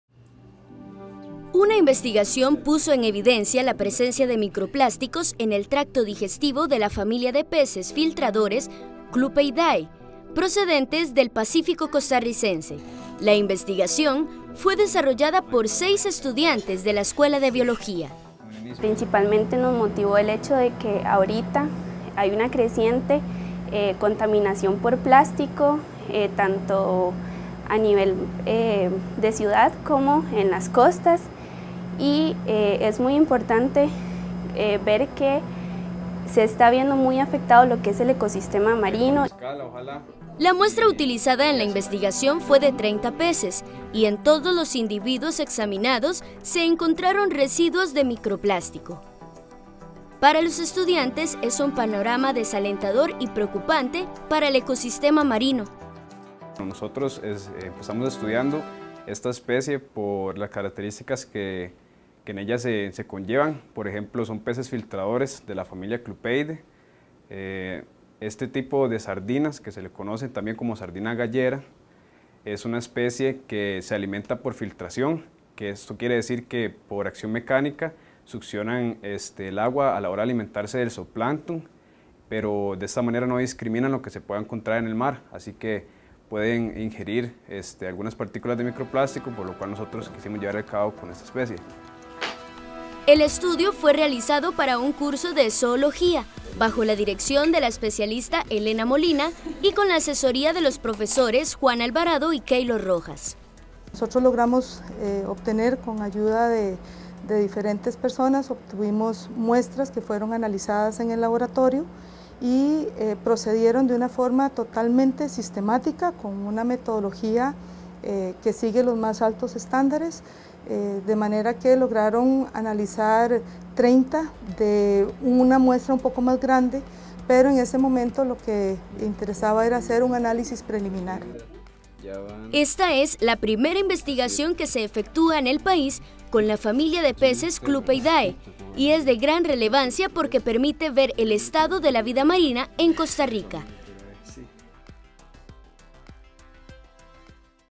Estudio de la UCR halla desechos de microplástico en peces del océano Pacífico.
Un estudio pionero, realizado por estudiantes de tercer año de la carrera de Biología de la Universidad de Costa Rica, puso al descubierto la presencia de microplásticos en peces del Océano Pacífico.